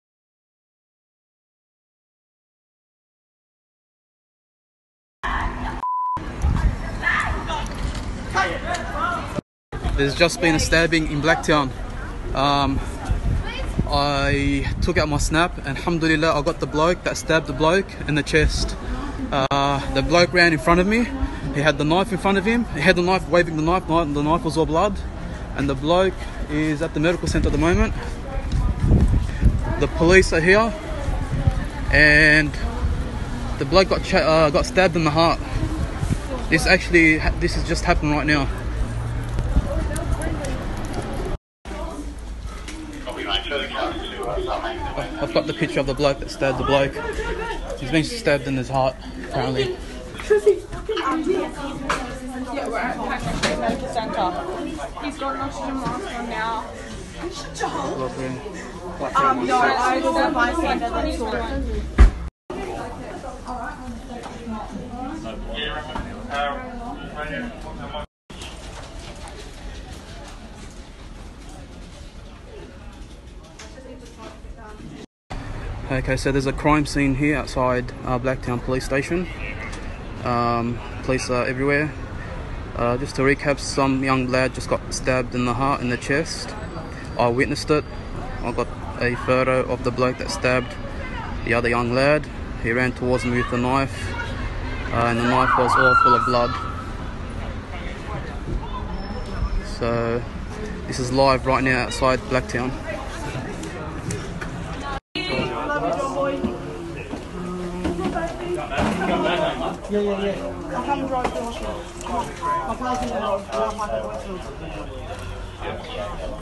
Teenager Injured After Reported Stabbing on Blacktown Street
Courtesy: Zaky Mallah